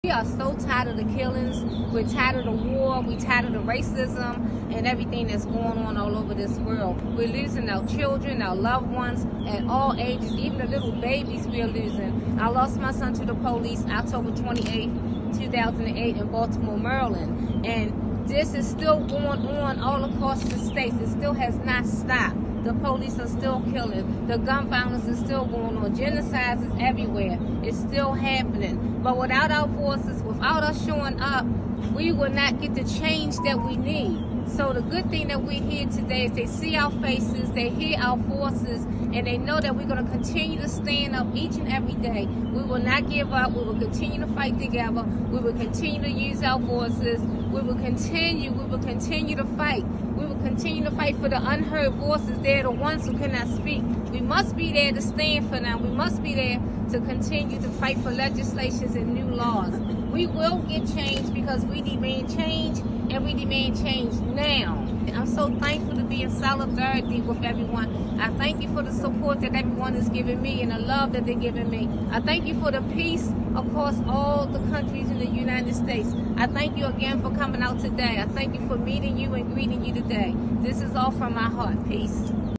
From Baltimore to Gaza, Stop Killing Our Kids!
From Baltimore to Gaza, stop killing our children!

This week, Darlene Cain, founder of Mothers on the Move, joined us in Congress to demand an end for support to the Gaza genocide. She shared how losing her son to the police in 2008 has impacted her as losing children is impacting so many mothers in Gaza right now.

The same system that kills innocent Palestinian children, women, and men does the same to our communities here in the United States. Our struggle for peace, justice, and freedom is directly linked to the struggle for Palestinian liberation.